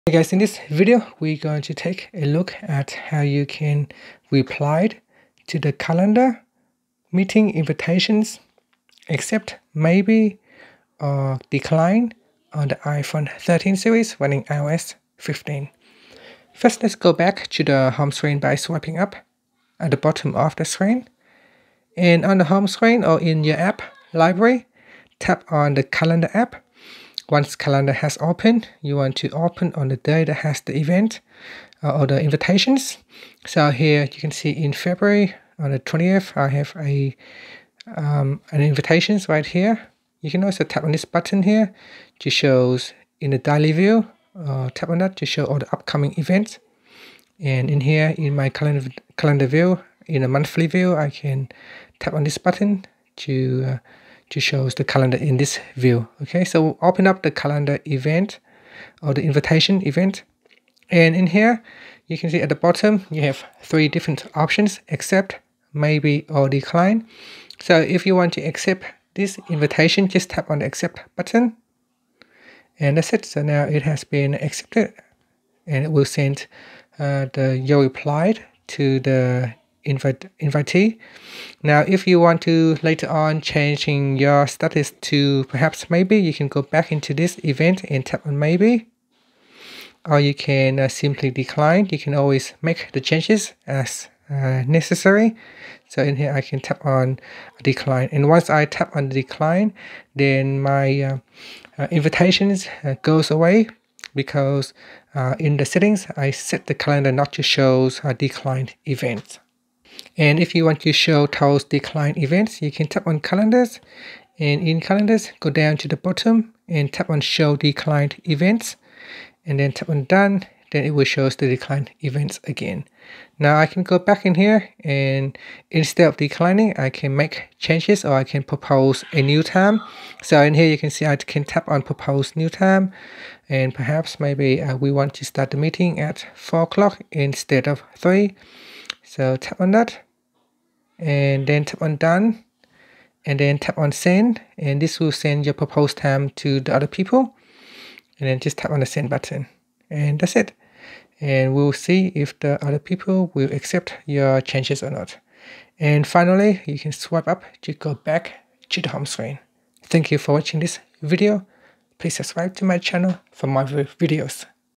iPhone 13/iOS 15: How to Reply To Calendar Meeting Invitations (Accept/Maybe/Decline)
Learn how you can reply to calendar meeting invitations (Accept/Maybe/Decline) on the iPhone 13 / iPhone 13 Pro/Mini.